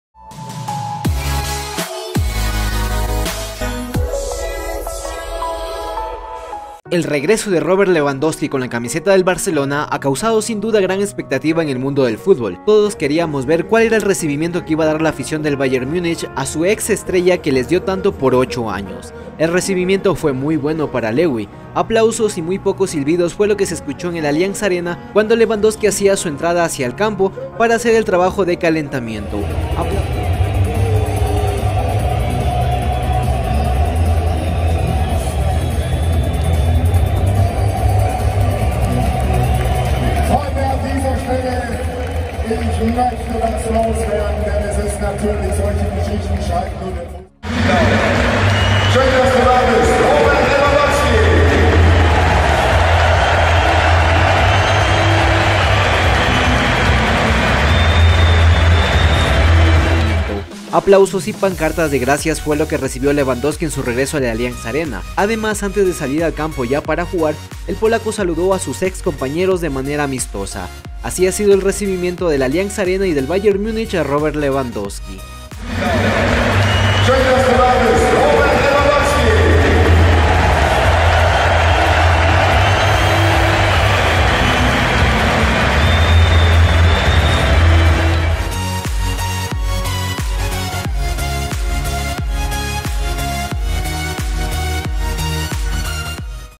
ASÍ RECIBIERON a LEWANDOWSKI la AFICIÓN del BAYERN MUNICH en el ALLIANZ ARENA
Había mucha expectación por ver qué recibimiento le tributaba el Allianz Arena a Robert Lewandowski, en su regreso a Múnich con la camiseta del Barça. Y aunque hubo algunos pitos, la ovación fue mayoritaria. El delantero azulgrana agradeció el cariño de la que fue su afición devolviendo los aplausos cuando salió a calentar.

La megafonía del Bayern anunció el once del Barça e hizo una pausa al citar a Robert Lewandowski, al que presentaron como ex jugador del Bayern y recordando los goles que había marcado con el club alemán en sus ocho temporadas en el club bávaro. Excepto los seguidores situados detrás de una portería, la mayoría del estadio le recibió con aplausos.

Era el primer regreso de Lewandowski a Múnich tras haber abandonado el Barça este pasado verano, después de ocho temporadas en el Bayern de Múnich.